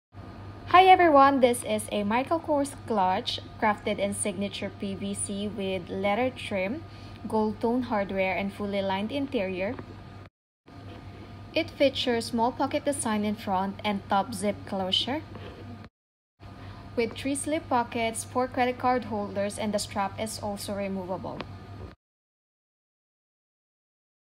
MICHAEL KORS Bedford Signature Clutch - Brown / Acorn 32F7GBFW8B-847
A Michael Kors Bedford zip clutch crafted in signature PVC with a leather trim, gold-tone hardware and fully lined interior. This Michael Kors clutch features a top zip closure, 3 slip pockets and 4 credit card slots; a removable leather wrist strap. Dimensions: 6.7" x 4.1" Michael Kors 32F7GBFW8B